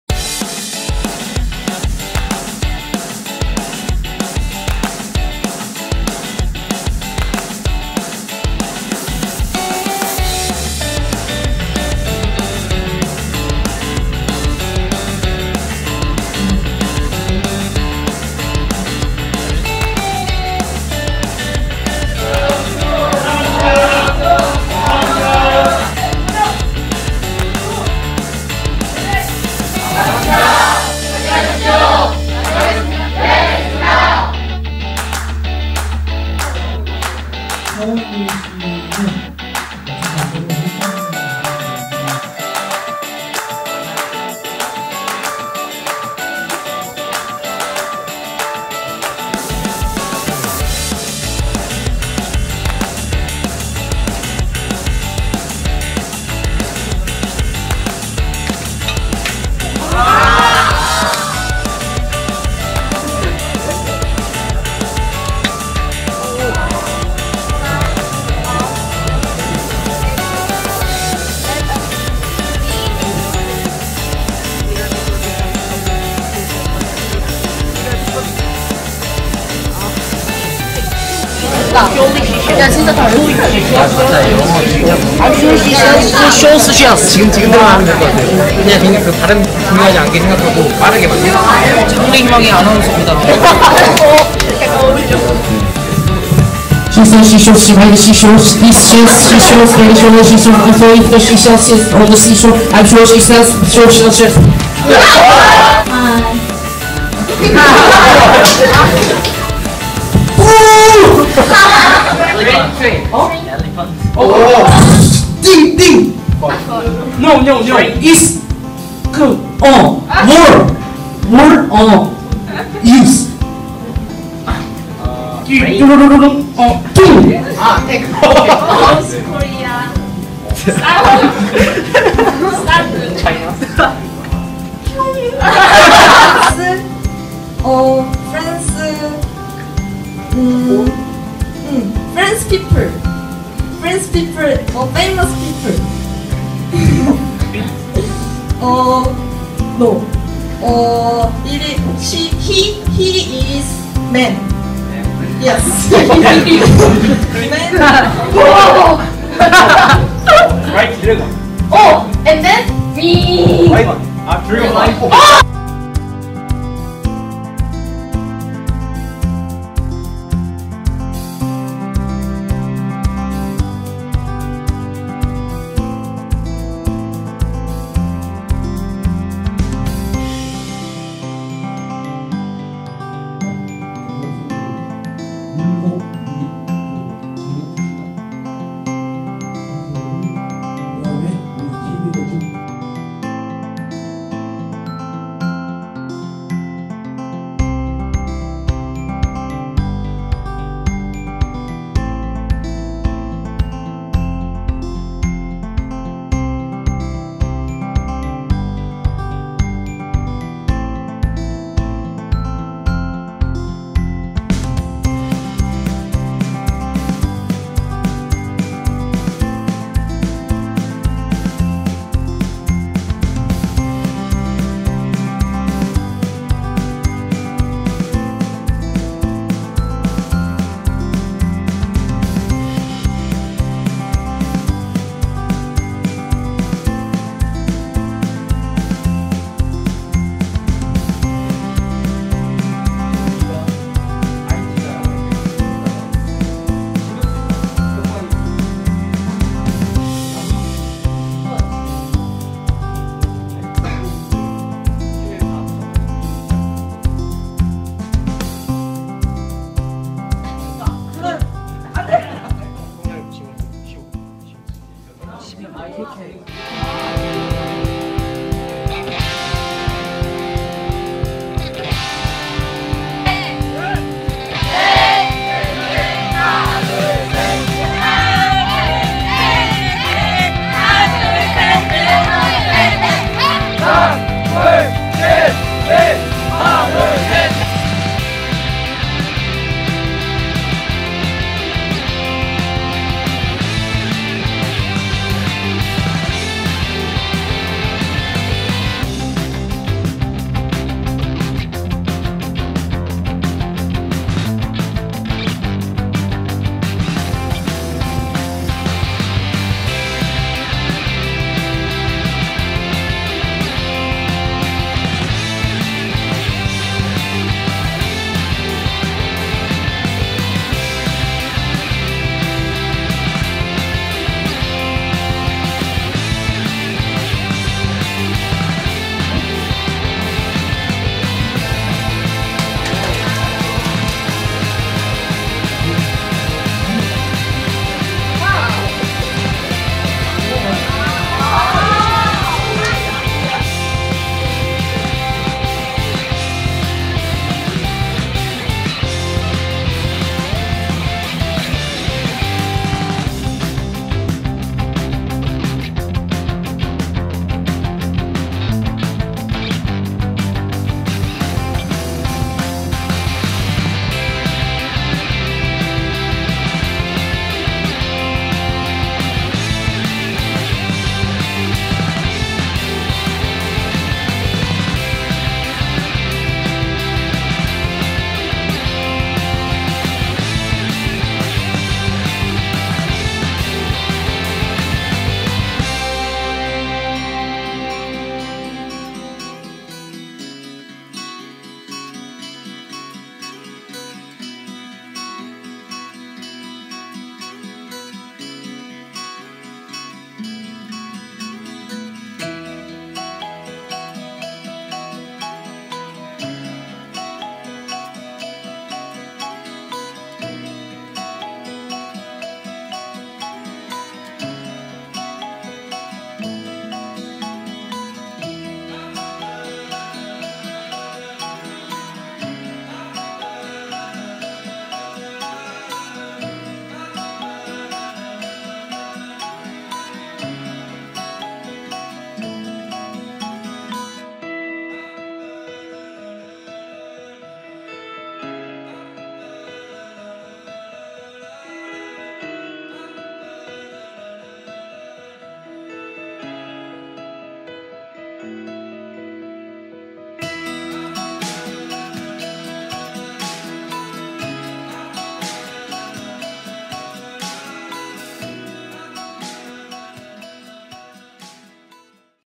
2017. 지리산고등학교 인성캠프
2017년 7월 28일~ 30일, 2박 3일간 전국에서 모인 중학생 36명은 지리산고등학교의 인성캠프에 참가하였습니다.

여러 가지 체험 학습과 인성 교육 프로그램을 통해 학생들은 한 뼘 더 성장하는 계기가 되었습니다.

앞으로도 지리산고등학교에 많은 관심 부탁드립니다.